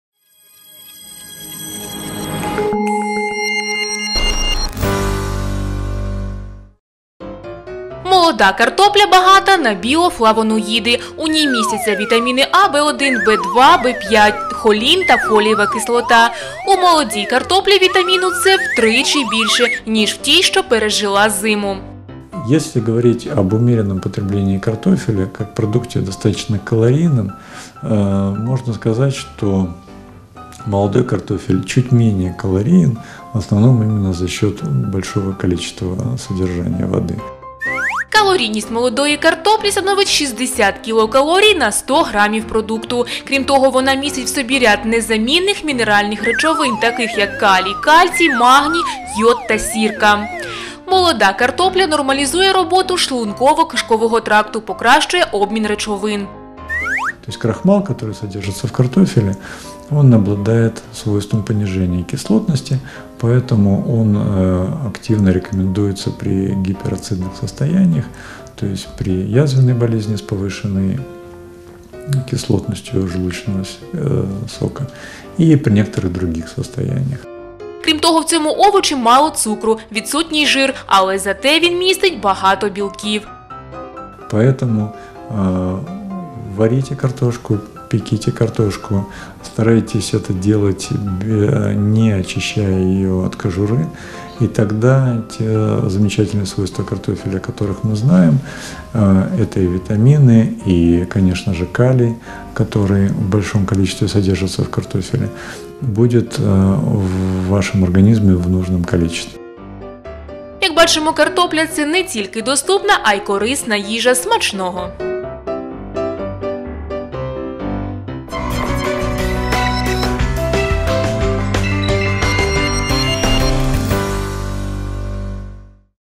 Картопля. Урок правильного харчування в рубриці “Їстівна азбука”
Усе про картоплю знає лікар-дієтолог Роман Мазо.